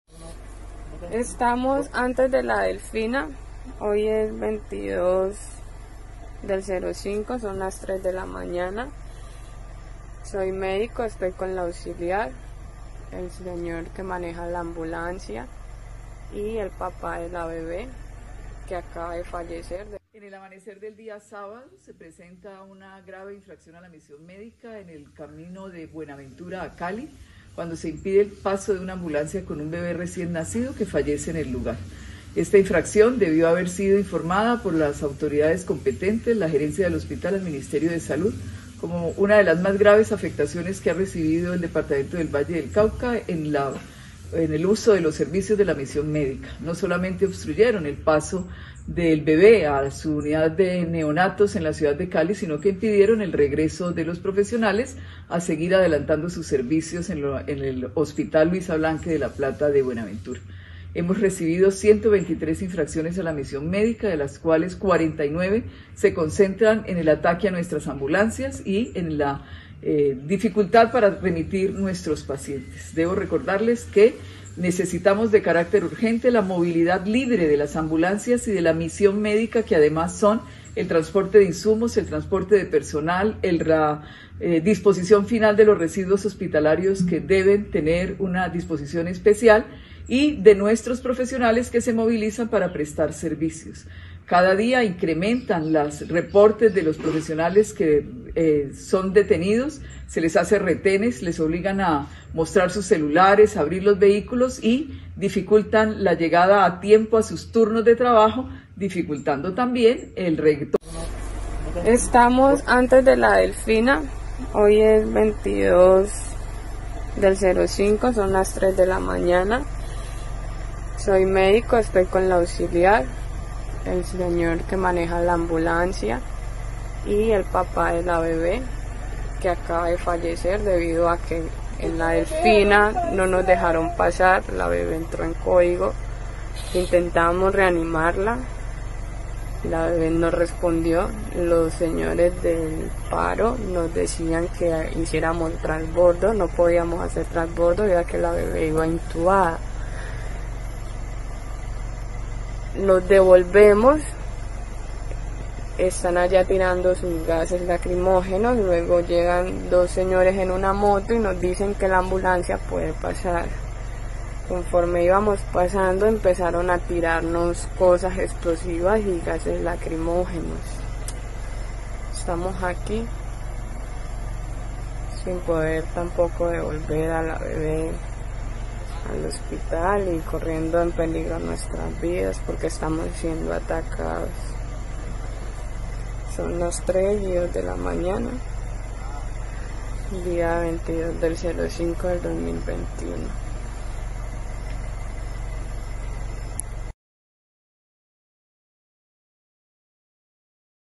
Caso de recién nacida que murió en una ambulancia, por bloqueo en Buenaventura no pudieron pasar
A las 12:30 reposa en la historia clínica la salida de la recién nacida en la ambulancia, tras un recorrido de cerca de 45 kilómetros hasta La Delfina llegaron a la 1:30 al punto de bloqueo, en el que comunidades indígenas “descansaban”, allí no se le dio paso inmediato al vehículo de asistencia médica, la Orivac reportó que  “el ataque del Esmad” fue a las 2:30 a.m., y en un video una de las médicas que iba en la ambulancia, revela que eran las 3:00 de la madrugada, ya la bebé había muerto y que seguían en el sitio porque estaban tirando “gases y elementos explosivos”.

Que sentían temor por su vida.

Lo que hasta ahora se ha conocido permite establecer que apenas la ambulancia llegó no tuvo el paso prioritario que ameritaba y que luego, por el accionar del Esmad para despejar y dar paso a la caravana de camiones de carga, no pudieron moverse.

Este punto de La Delfina y otros de la vía a Buenaventura han estado bloqueados varios días del paro nacional.

Sin embargo, ni desde el hospital del puerto, ni secretaria de Salud del Valle han señalado con claridad quién impidió el paso de la ambulancia.

TUBARCO ha intentado contactar con la ORIVAC, para establecer una versión de su parte sobre este particular, y no ha sido posible. Solo se informó que están en “asamblea permanente”.

Los detalles del caso👇🏼

*Actualización.
La noche de este domingo la Asociación de Cabildos Indígenas del  Valle del Cauca Región Pacífico, se pronunció y lamentaron lo ocurrido con la bebé. Pidieron respeto a las misiones médicas.

Señalan que a la ambulancia si se le dio paso, “al regreso se encontró con la arremetida del Esmad con gases lacrimógenos y disparos en la vía”.